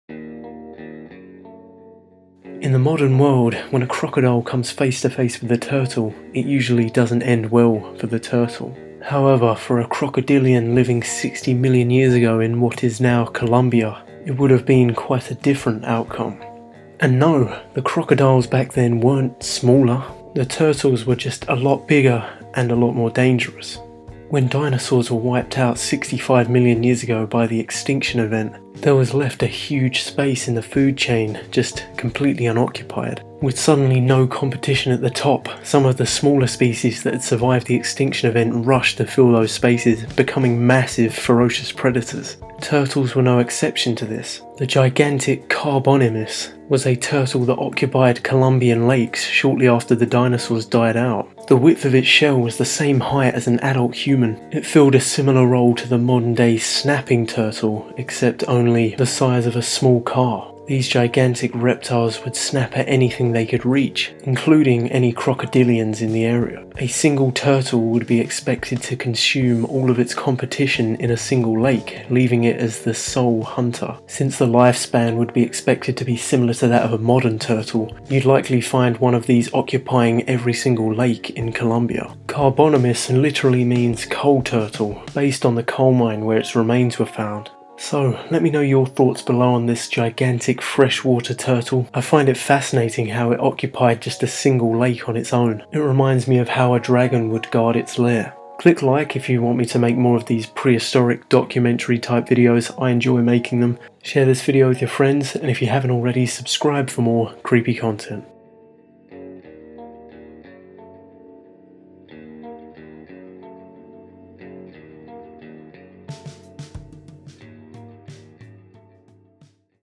The Giant Prehistoric Killer Turtle That Hunted Crocodiles | Carbonemys
Carbonemys, the prehistoric turtle that hunted crocodiles. A giant freshwater turtle that lived in Columbia 60 million years ago. It was the size of a small car, with a shell as wide as a human is tall.